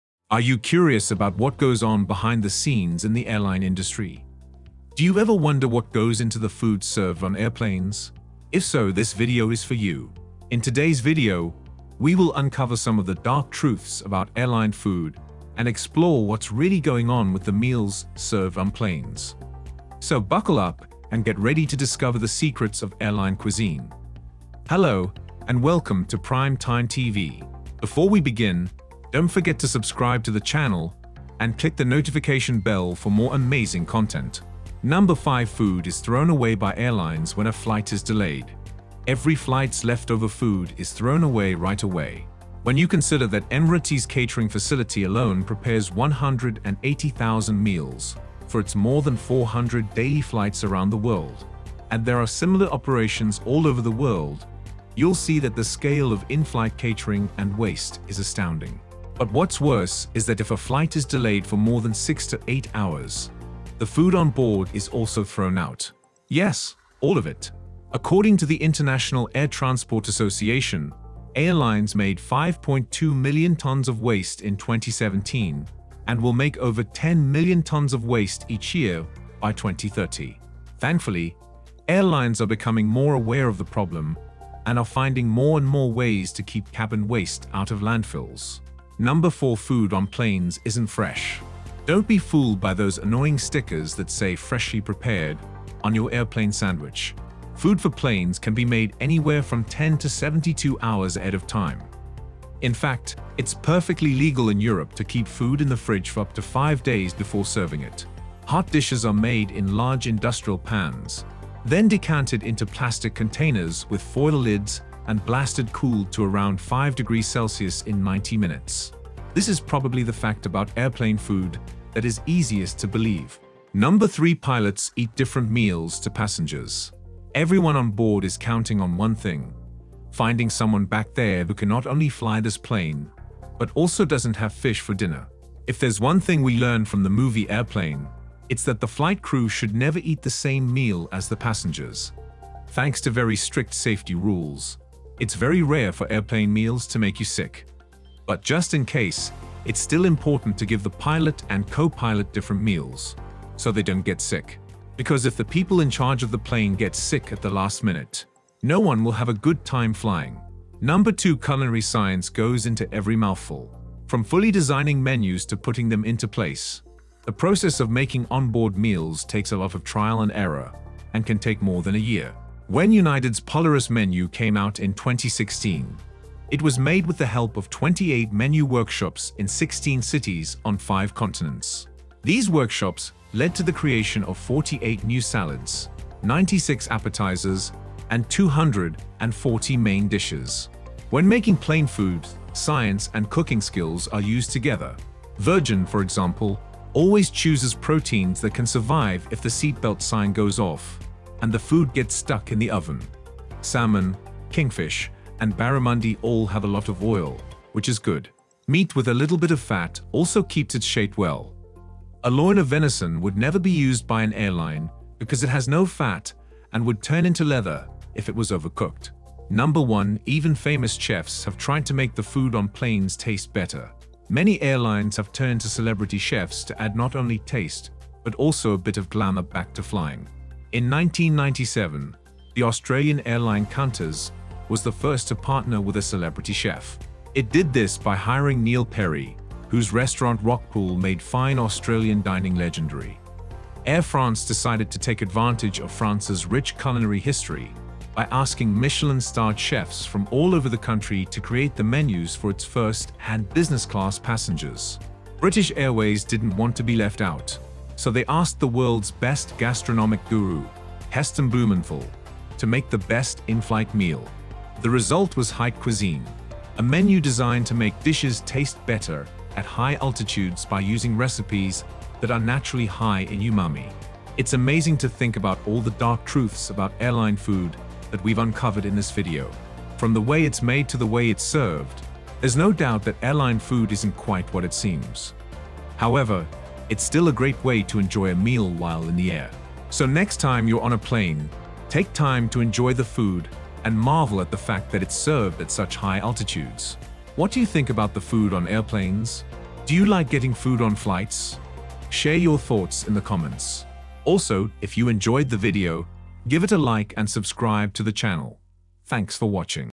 Dark Truths About Airline Food!
Are you curious about what goes on behind the scenes in the airline industry? Do you ever wonder what goes into the food served on airplanes? If so, this video is for you! In today’s video, we will uncover some dark truths about airline food and explore what's happening with the meals served on planes. So buckle up and get ready to discover the secrets of airline cuisine! Hello and welcome to Prime Time TV.